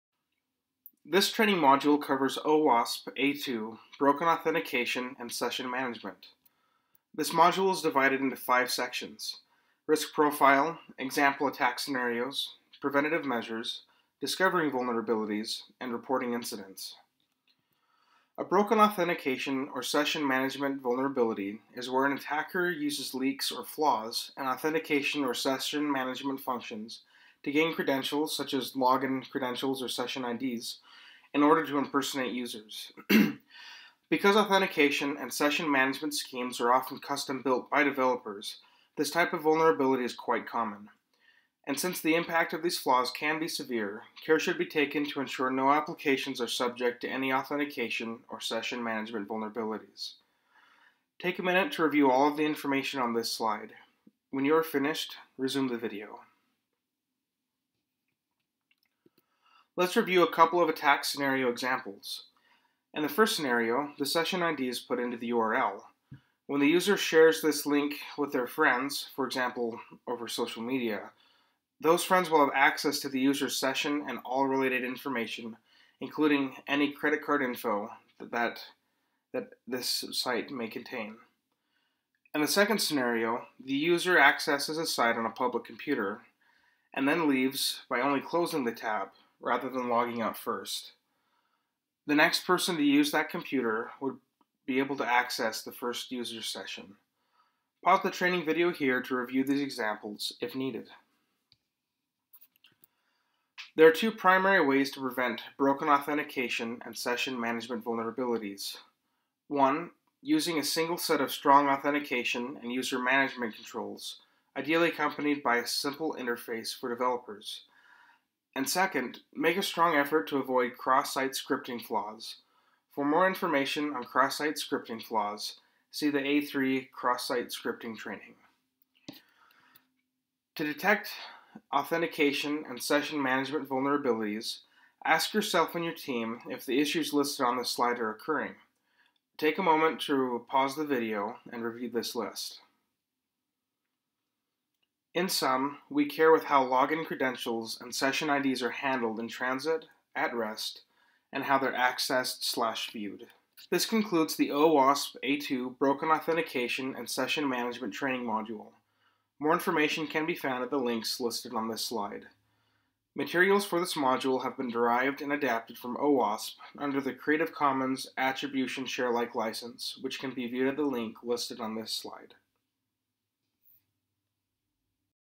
OWASP A2 - Broken Authentication and Session Management
This is video 2/10 covering OWASP's Top 10 Most Critical Web Application Security Risks.